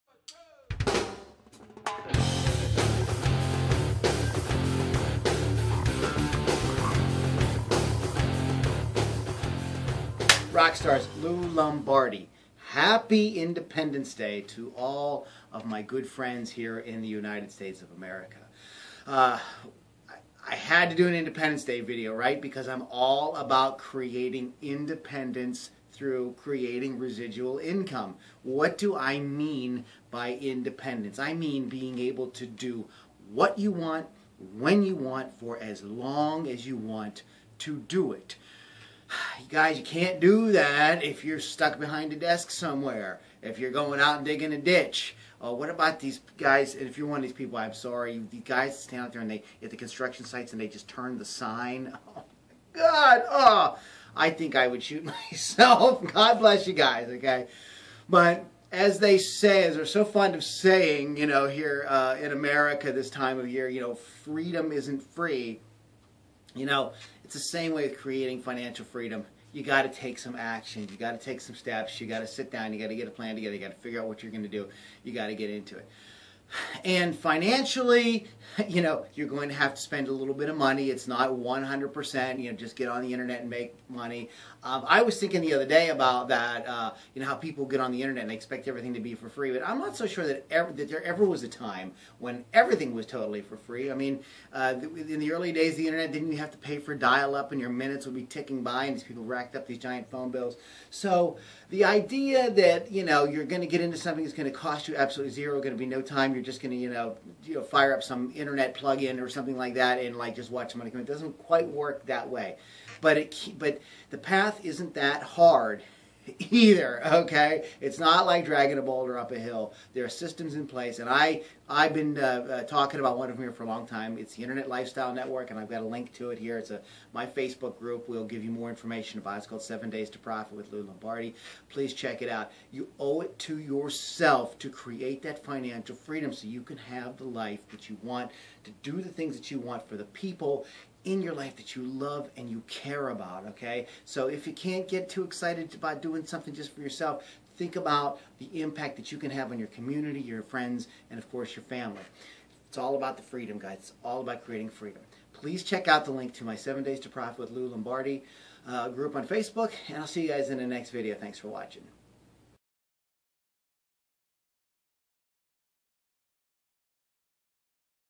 Internet Lifestyle Network Training: Happy Independence Day
Happy Independence Day! I am all about creating independence through creating residual income. Being able to do what you want, when you want, for as long as you want. You can't do that stuck behind a desk. You have to take action. You have to spend money to make money. The path isn't that hard. There are systems in place, like The Internet Lifestyle Network. You owe it to yourself to have financial freedom.

For Internet Marketing Made Simple, check out the Viral Success Network

lou lombardi, viral success network, internet lifestyle network, financial freedom, residual income, Independence Day